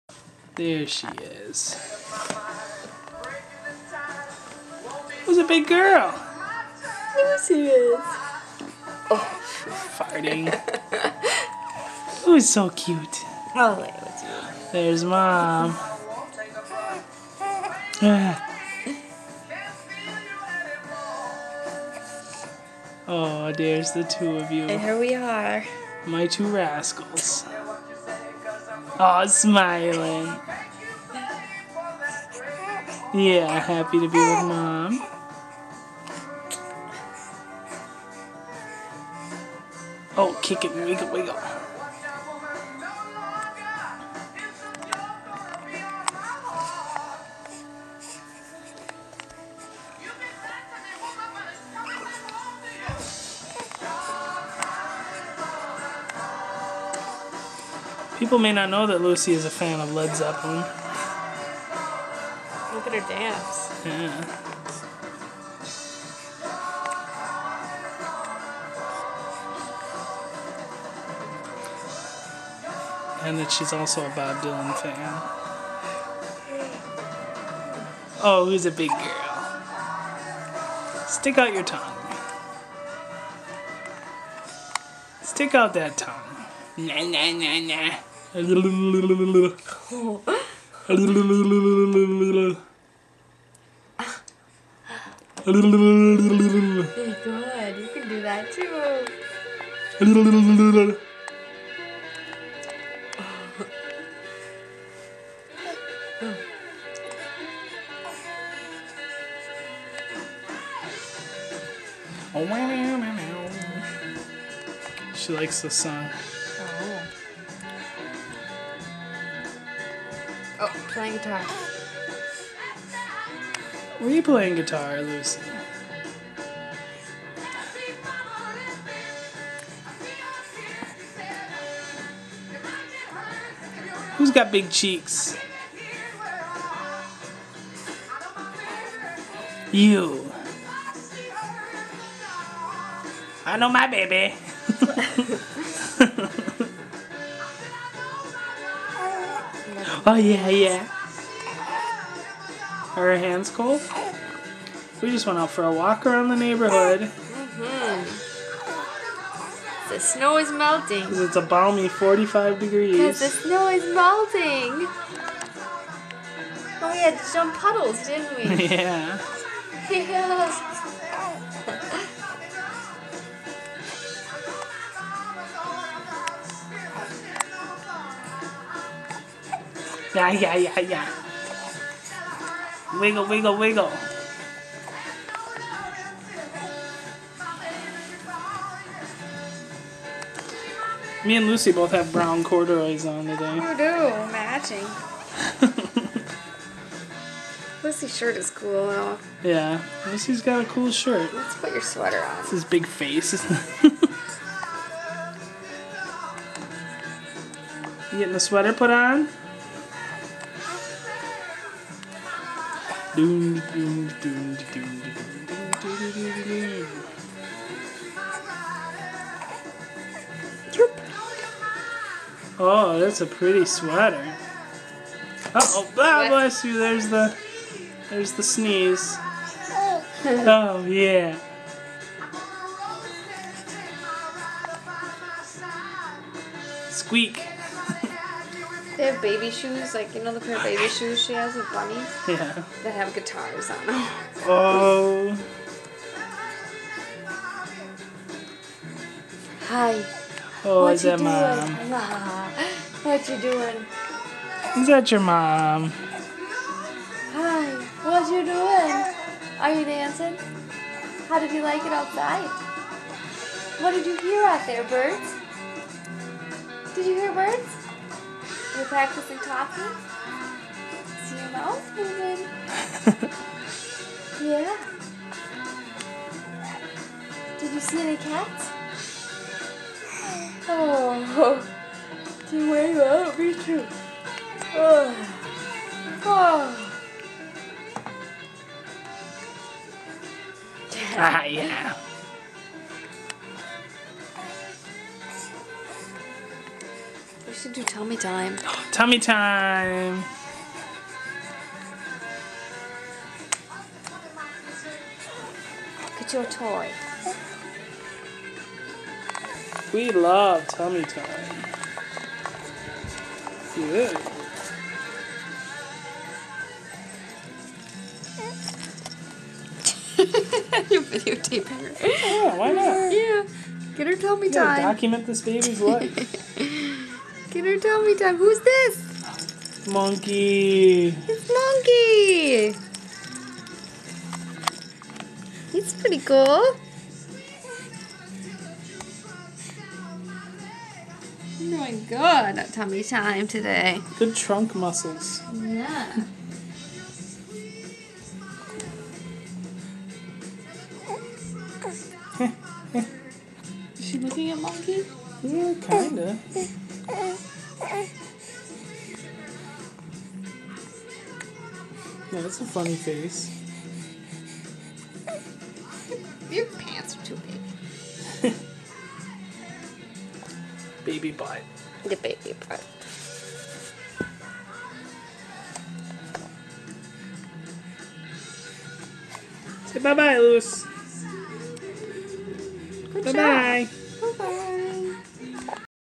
Lucy and Mom on the floor
This afternoon we went for a walk outside and after that I took this video. CUTE.